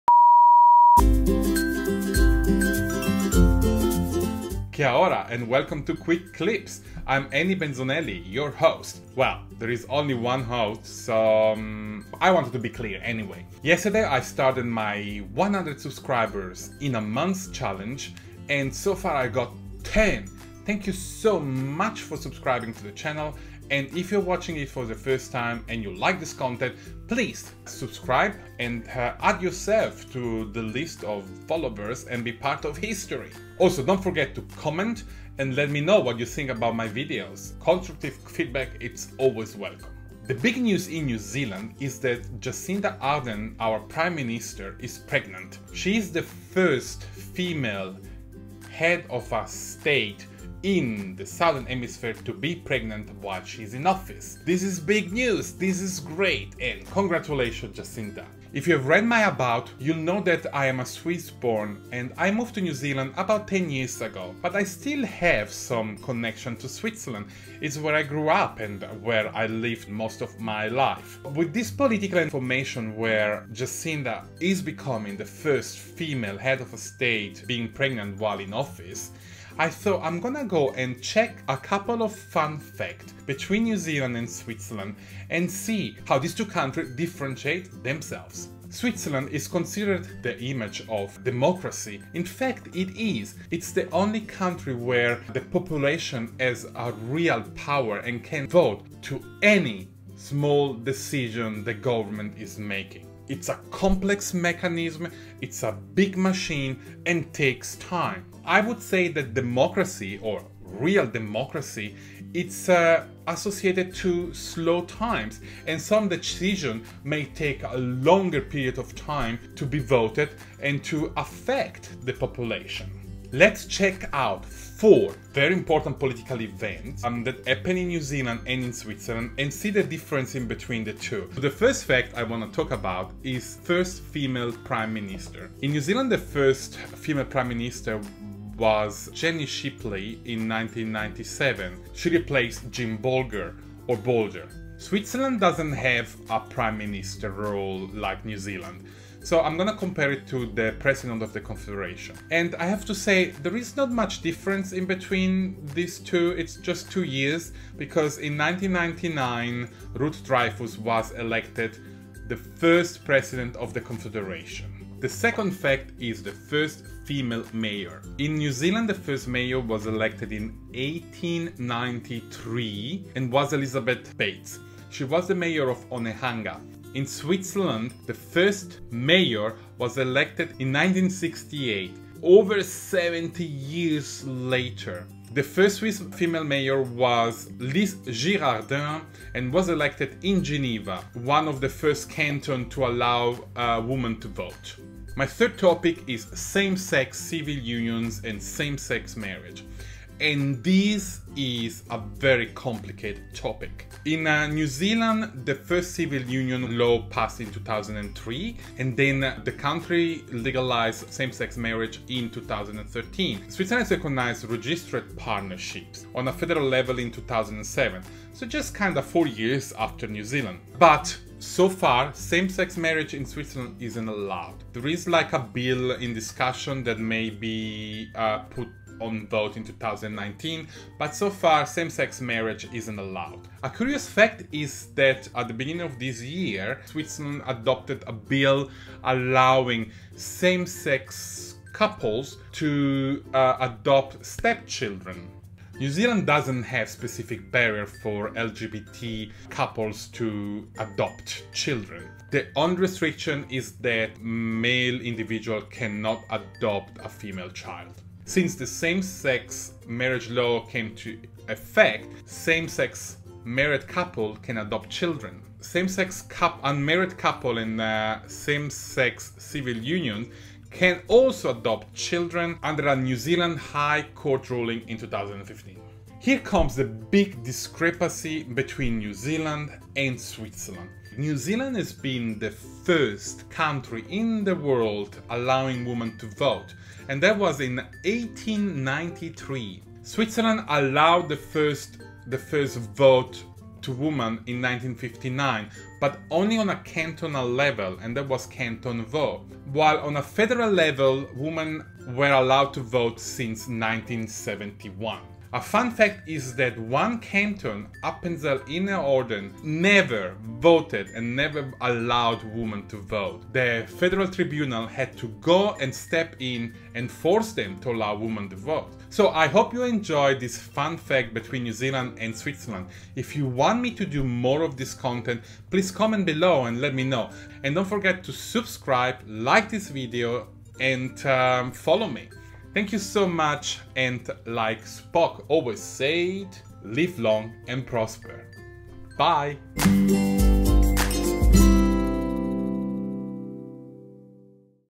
PM Jacinda Ardern's Pregnancy: Political Events that put New Zealand on the Map
Prime Minister Jacinda Ardern's Pregnancy: Political Events that put New Zealand on the Map. The Prime Minister of New Zealand has announced to be expecting her first baby by June 2018. How exciting. She is the first Prime Minister in the Southern Hemisphere to be pregnant while in office. I thought I bring you some fascinating fact that New Zealand had happened before my motherland Switzerland. It's a fun video, and hopefully, I'll be able to show you some interesting points.